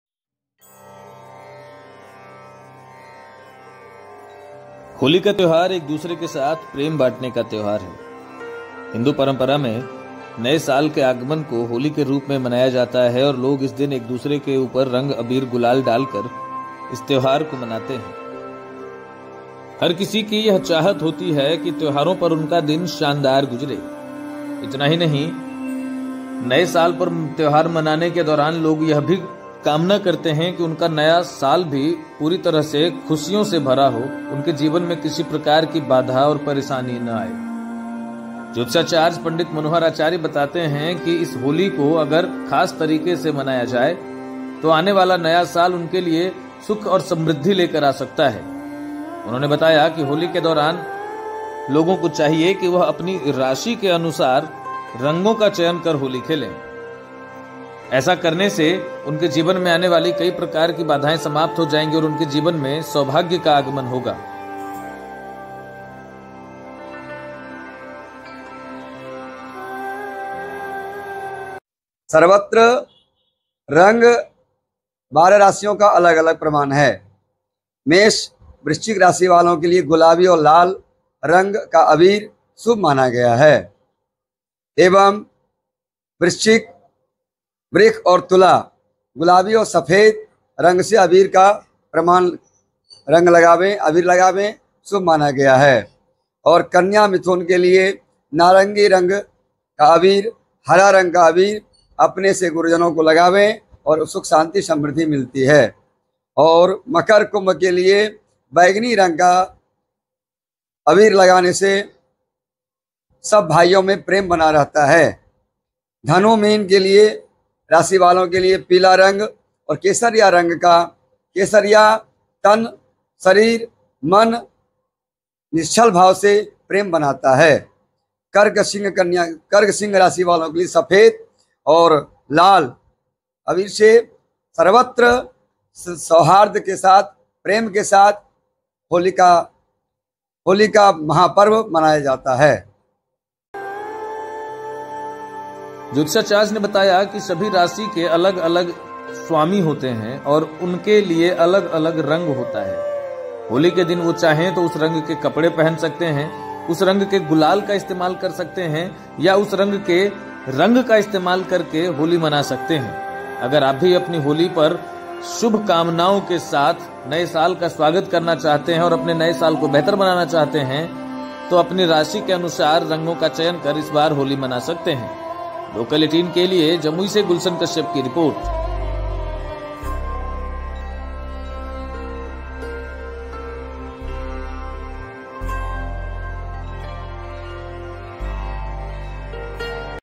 Holi Horoscope : राशि के अनुसार रंग चुनकर खेलें होली, जीवन में बिखर जाएगा खुशियों का रंग
Holi Horoscope: Holi is a festival of sharing love with each other. Every person wishes to have a good day on the festival. Not only this, the festival of Holi is celebrated to commemorate the arrival of Hindu New Year.
Holi Horoscope: होली एक दूसरे के साथ प्रेम बांटने का त्यौहार है. हर व्यक्ति की यह इच्छा होती है कि त्योहार पर उनका दिन अच्छा हो. इतना ही नहीं होली का त्यौहार हिंदू नव वर्ष के आगमन के उपलक्ष्य में मनाया जाता है.